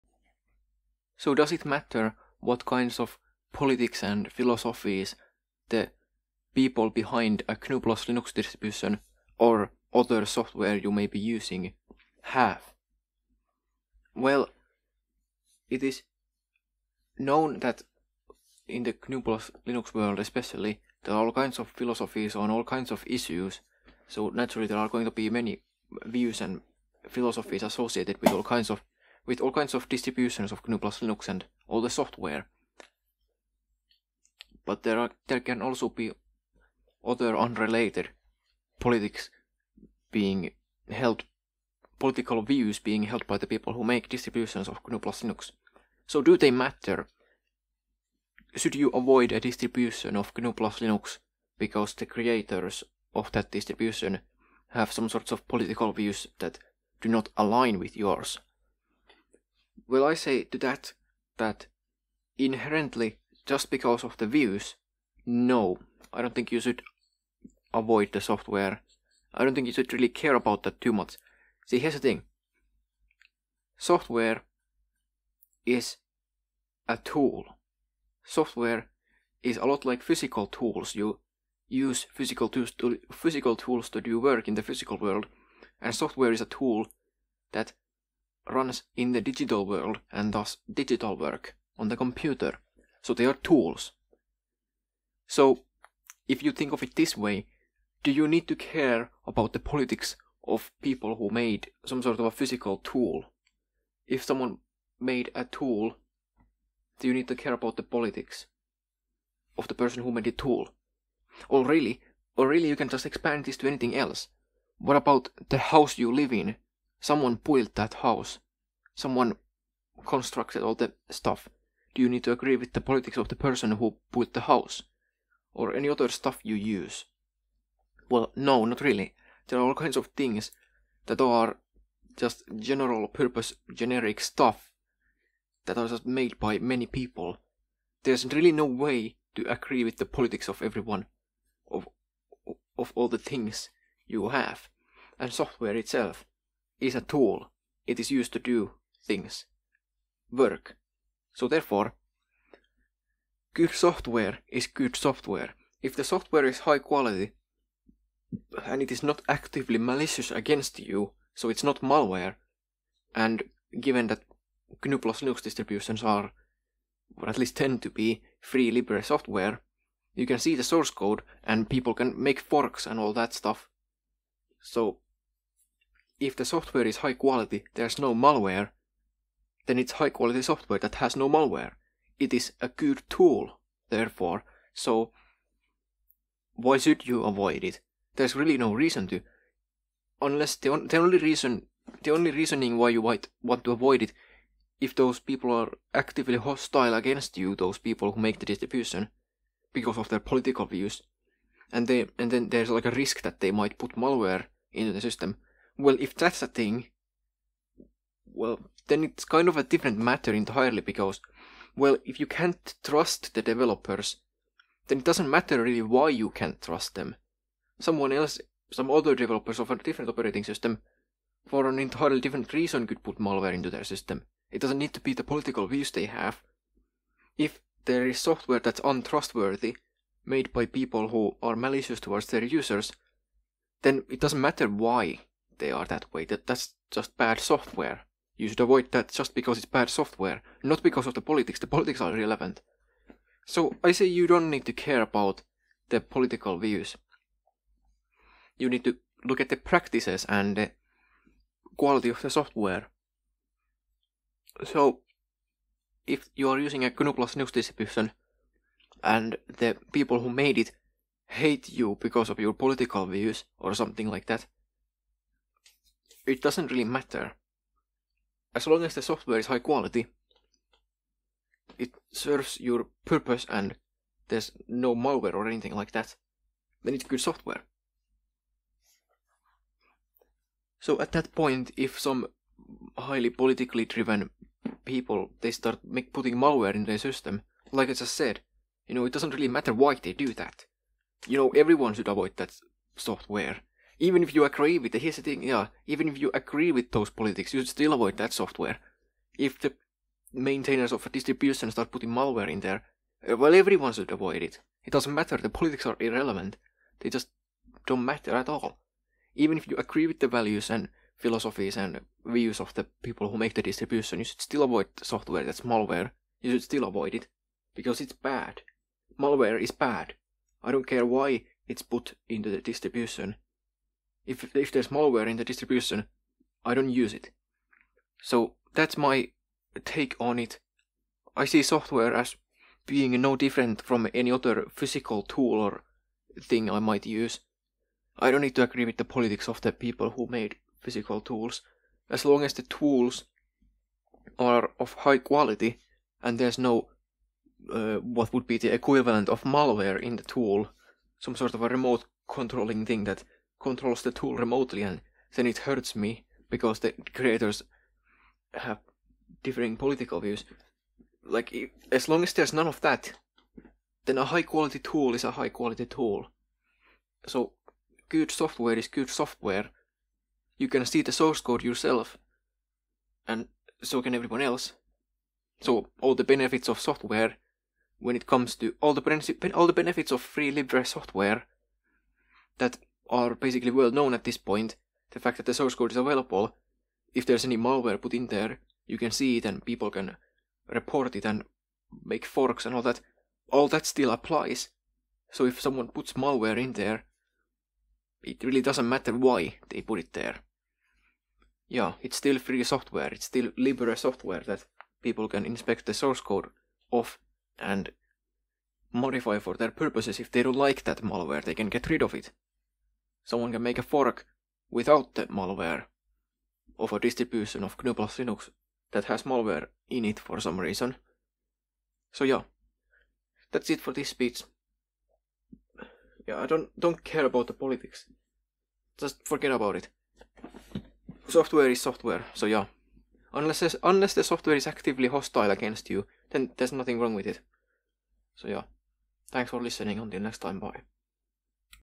Do You Need to Care about the Politics of the People who Make the Software You Use?
I say no.

Free/Libre software is software that respects the user's freedom. Software that meets the "4 Freedoms" is considered free/libre. The free software movement was started by Richard Stallman to to pursue the liberation of cyberspace. To create a free digital world for computers users.
Richard Stallman started the GNU project to develop an operating system for this, and pioneered the concept of copyleft with their GNU GPL license. Copyleft is a specific kind of a free/libre license that guarantees the user's freedom is preserved when the software exchanges hands.